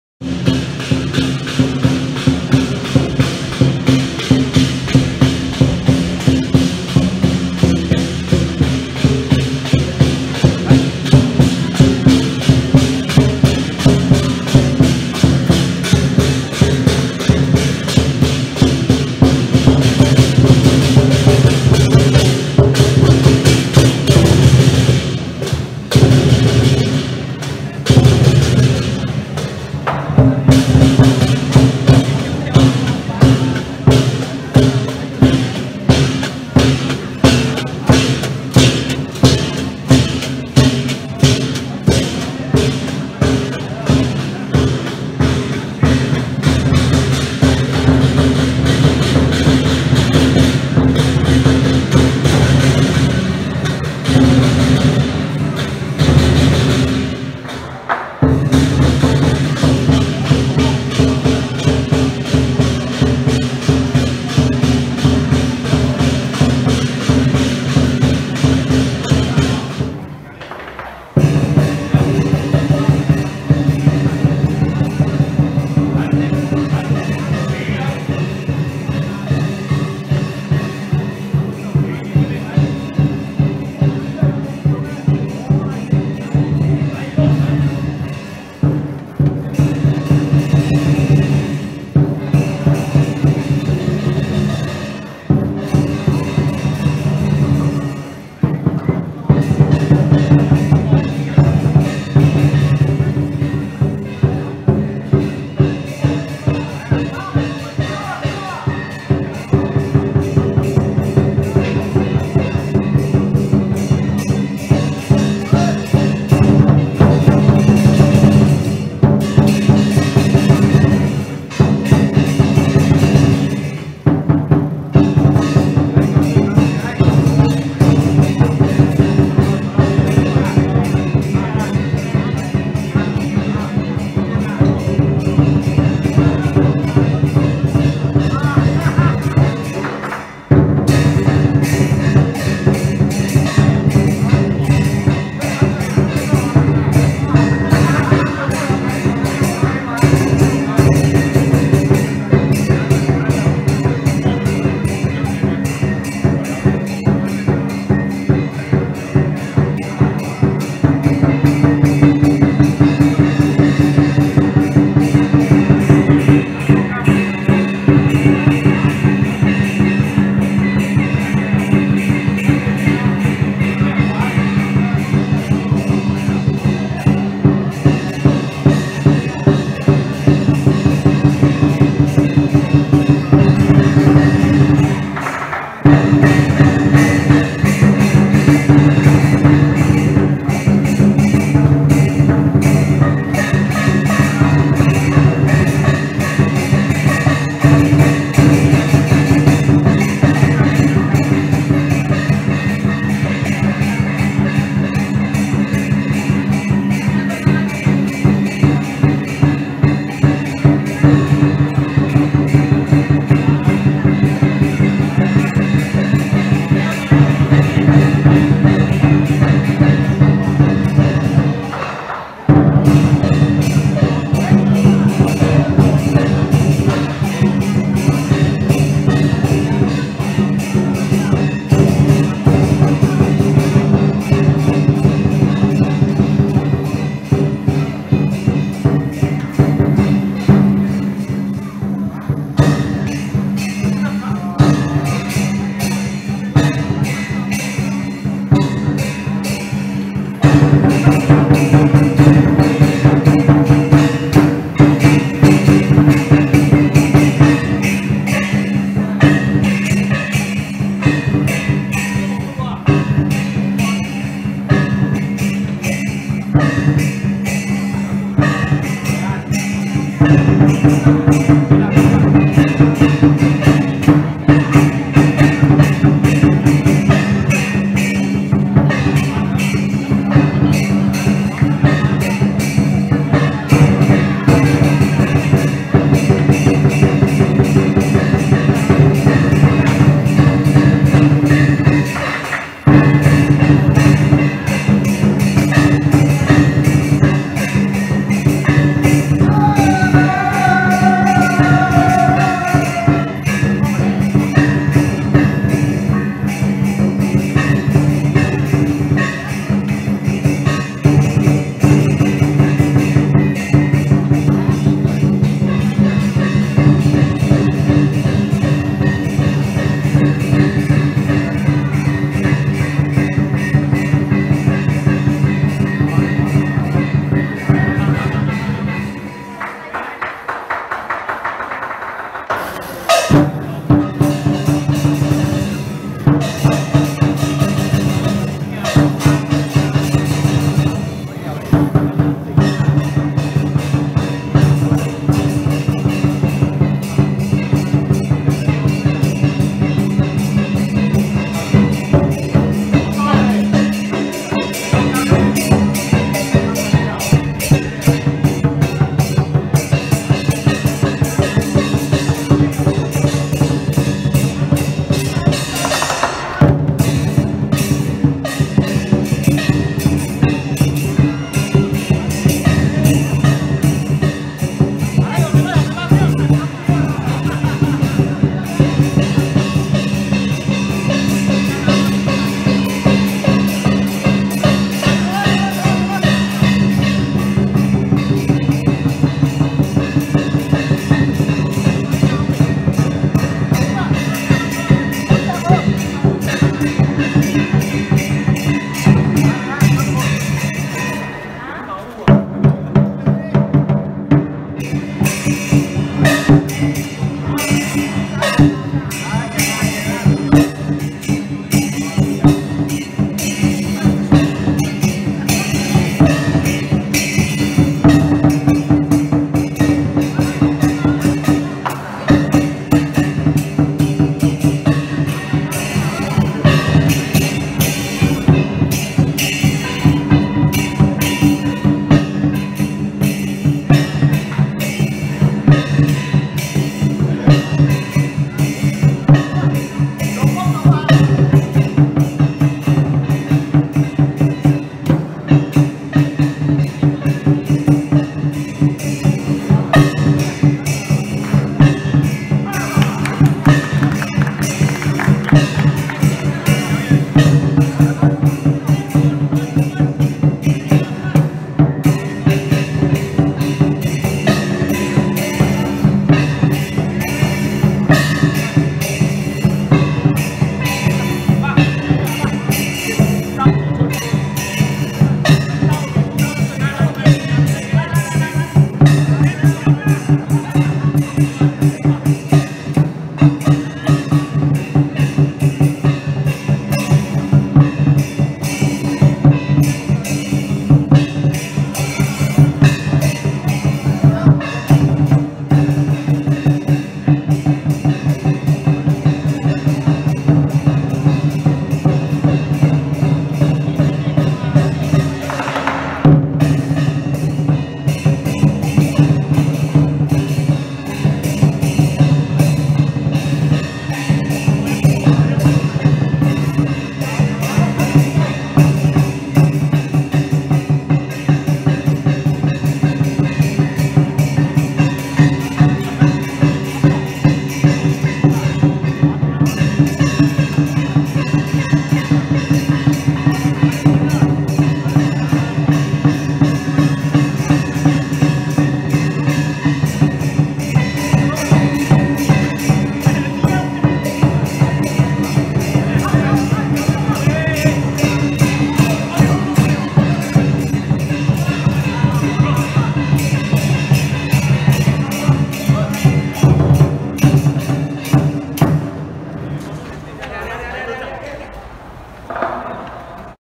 合衆龍獅文化團与玉封聖佛壇龍獅學院 Hup Zhong And Yu Feng Sheng Fo Tan Arrived at Shan An Tan 31 July 2022
Event: Shan An Tan Yew Keng

Date: 31 July 2022

Venue: Shan An Tan